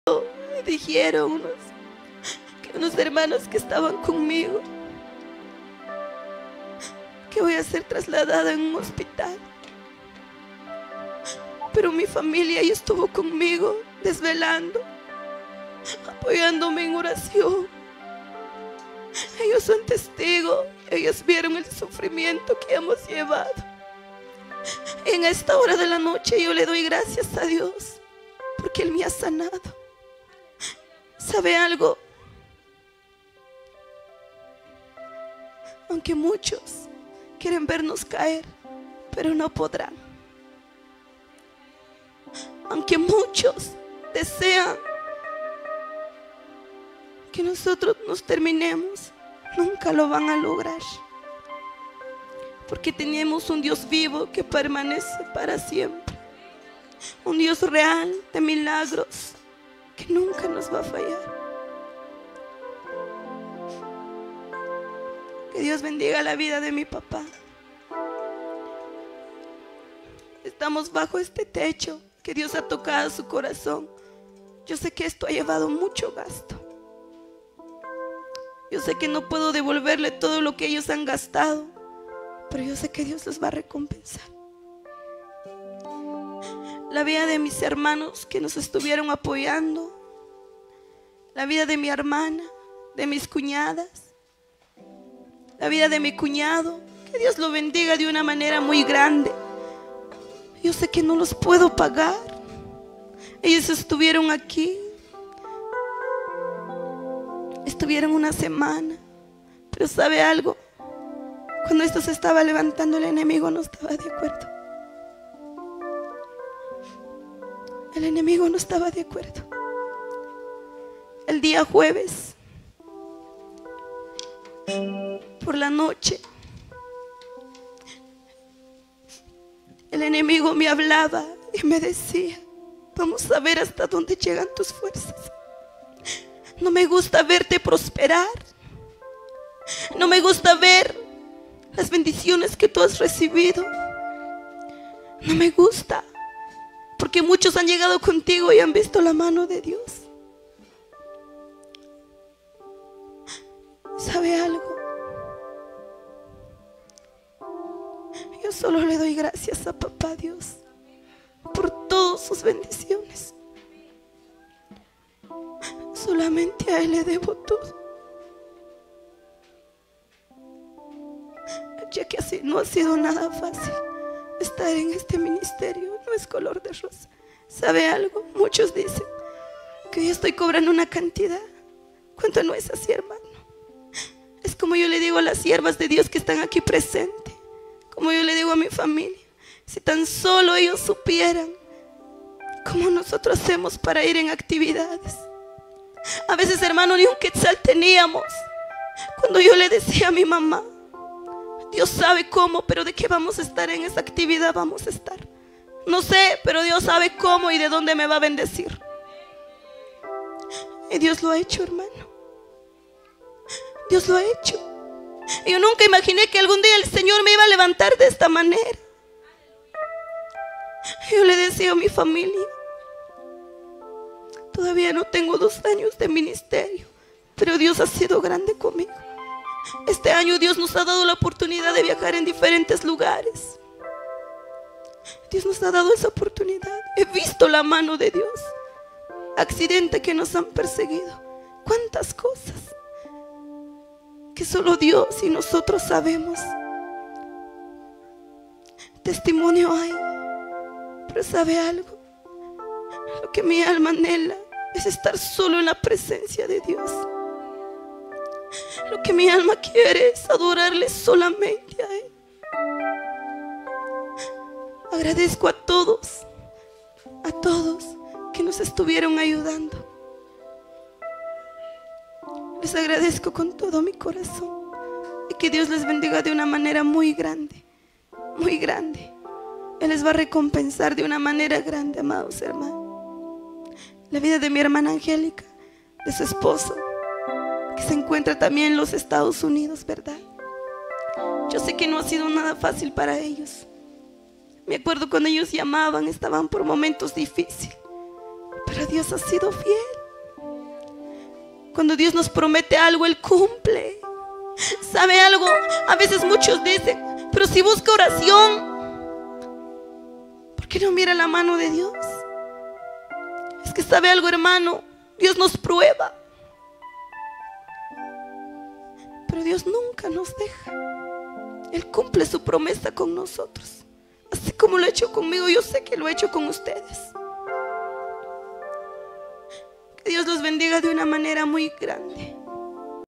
Tremendo testimonio de la solista Gloria Lopez. mira este video
tremendo testimonio de la solista Gloria Lopez aquí te lo presentamos en Alfa y Omega tv canal 7